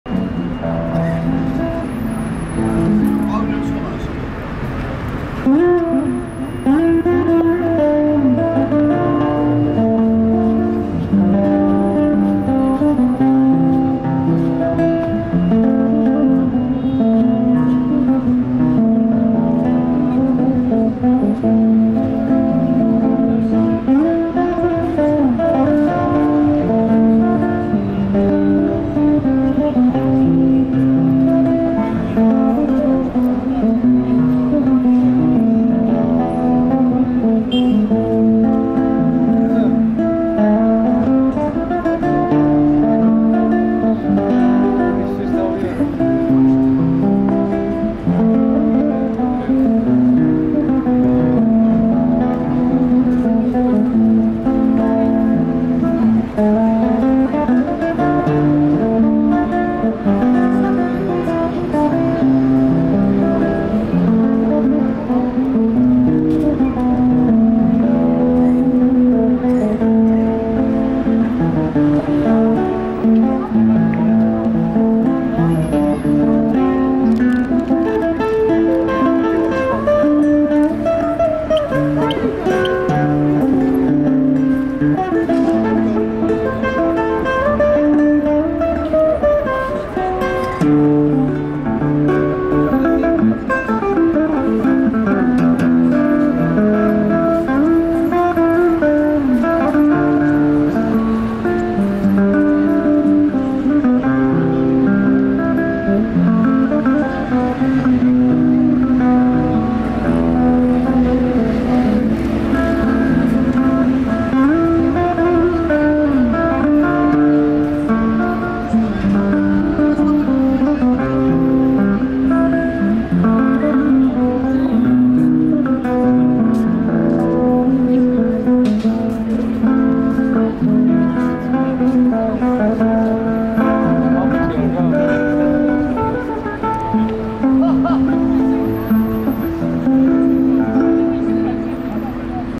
Young Man Asks An Old Street Musician To Perform Georgian Music | Tbilisi Street Music
Donation for channel development Young Man Asks An Old Street Musician To Perform Georgian Music | Tbilisi Street Music Amazing Discount!
- JBL Vibe 200TWS
- JBL CHARGE 5
- Beats Solo3 Wireless